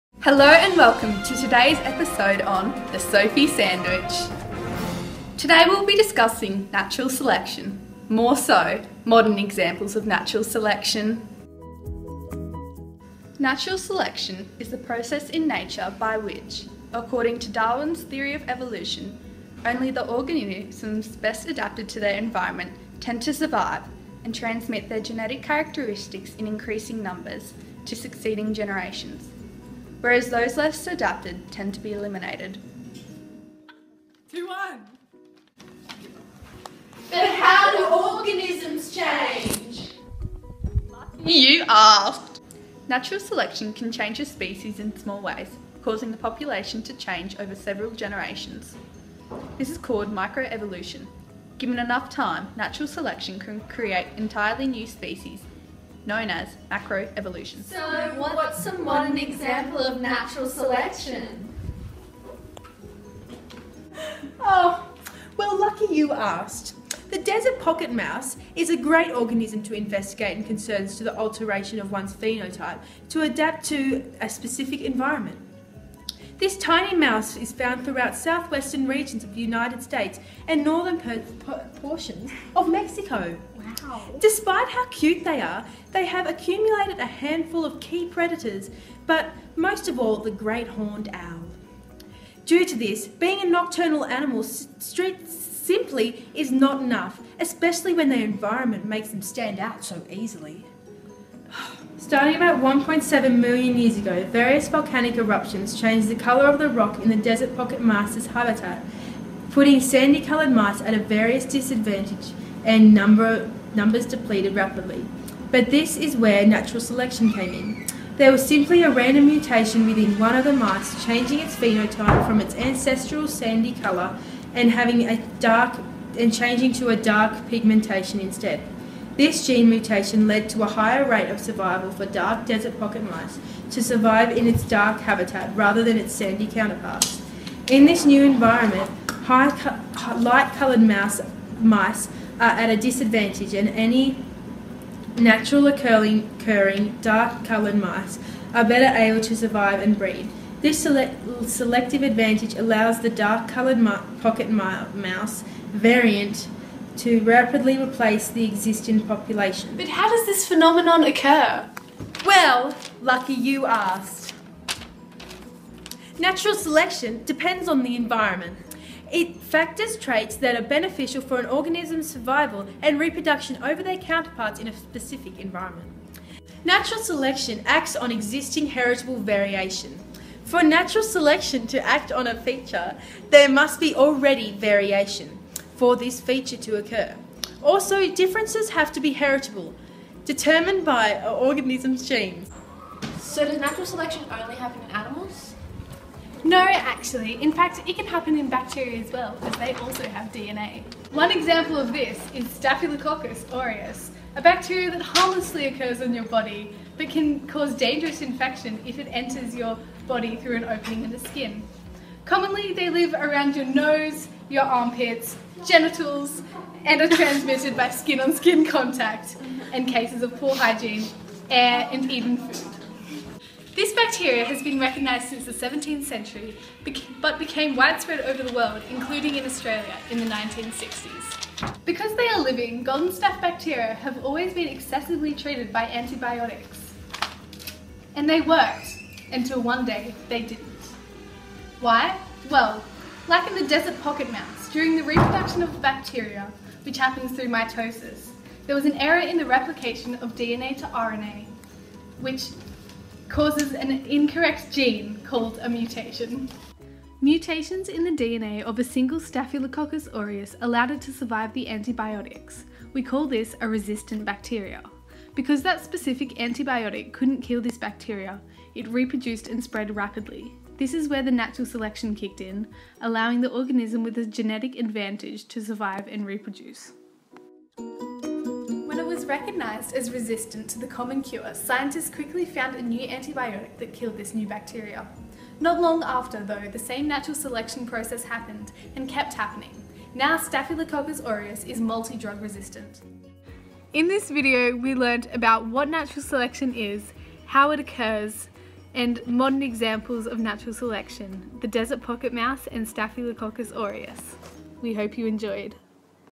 Modern Example of Natural Selection
A short video made by 2018 HSC Biology students.